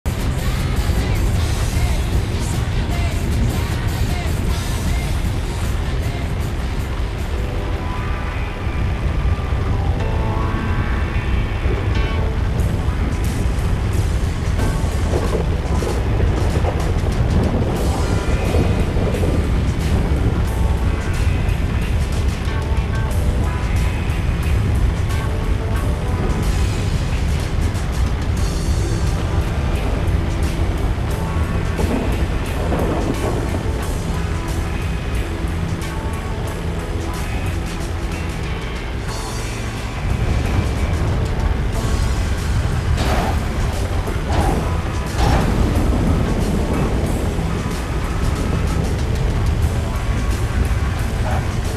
LANDING PLANE ON A RAINY DAY
Landing a Stunt Plane on a Rainy Day. Trevor Madness.